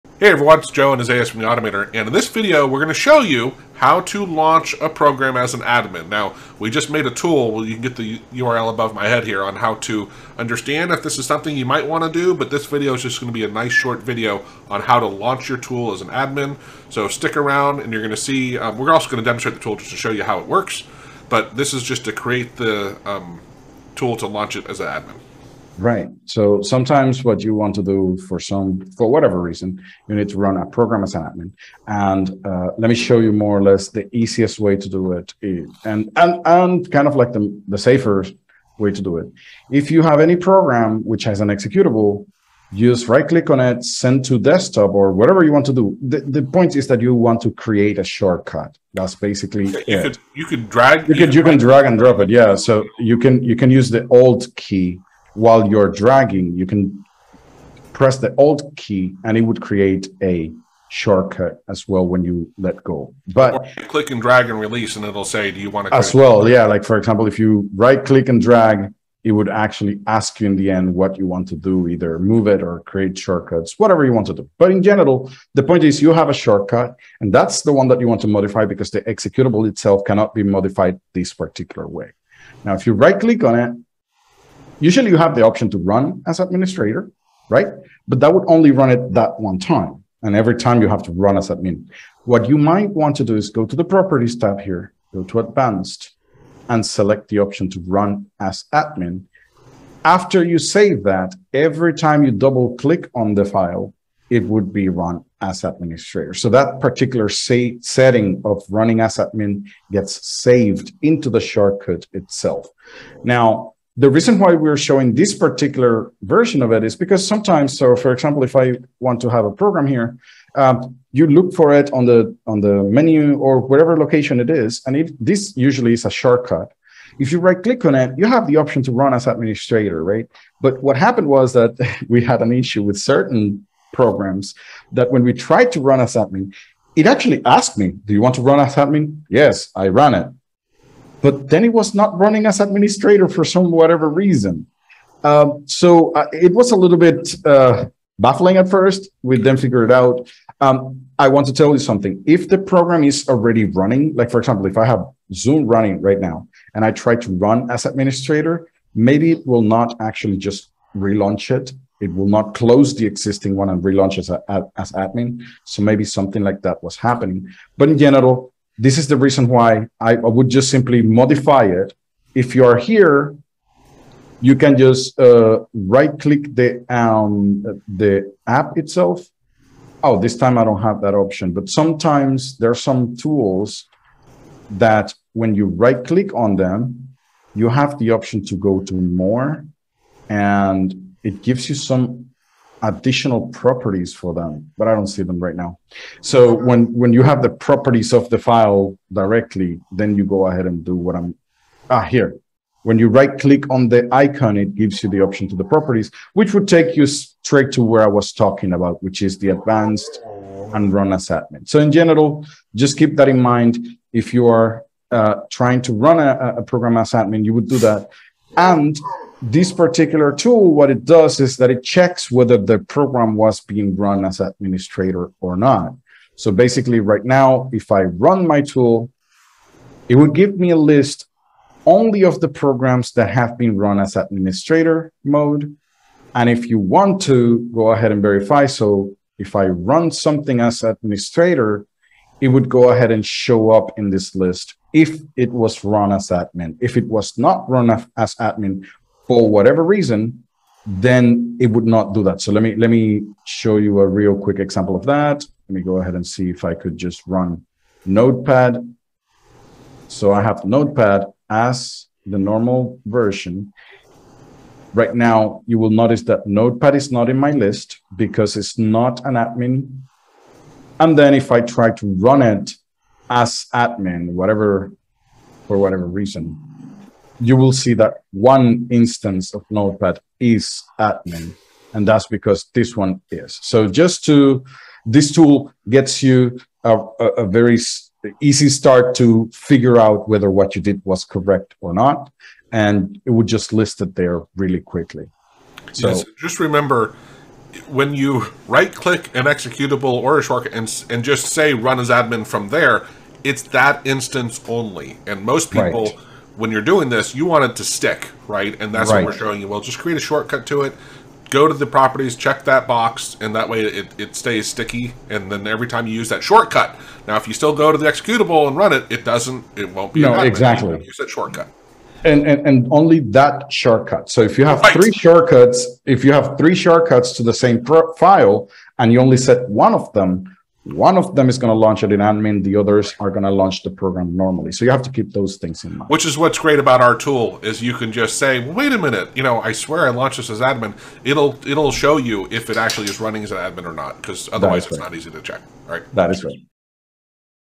🤴 Be the king of program execution - Force programs to run at admin level!
Here we show you how you can create a shortcut to a script/program and always have it run at admin level.  It is tied to our tool which helps you see what programs are being run as admin You can get that script

Summary: Right-clicking an executable or shortcut gives access to its properties to run a program with administrator privileges.

1. 00:00 🚀 Create a tool and right-click on the executable to launch a program as an admin.
 1.1 Launch a program as an admin by creating a tool and right-clicking on the executable.

2. 00:47 📌 Create shortcuts quickly by dragging and dropping with the alt key.
 2.1 You can create a shortcut by dragging and dropping with the alt key, or right-clicking and selecting an option.

3. 01:25 🔑 Run shortcut as admin to save setting for future use.
 3.1 Right-click on the shortcut and select the option to run as administrator to save the setting for future use.

4. 02:11 🤔 Restarting a program as an admin may not work as expected.
 4.1 Trying to run a program as an administrator may not close the existing one and relaunch it as admin.

5. 02:47 🔧 Right-click to modify.
 5.1 Right-click the app to modify it.

6. 03:00 💻 Right-clicking a file icon gives access to its properties to run an assignment.
 6.1 Right-clicking on a file icon gives the option to access its properties, which can be used to run an assignment.

7. 03:41 🔑 Running a program with administrator privileges will appear in a list, without won't.
 7.1 Running a program as administrator will show up in a list, while running it without administrator privileges will not.

8. 04:41 🔒 Tick the "Run as administrator" box in the properties of a shortcut to make it always run with admin privileges.
 8.1 Create a shortcut to an executable, go to the properties and check the box to make it run as admin every time.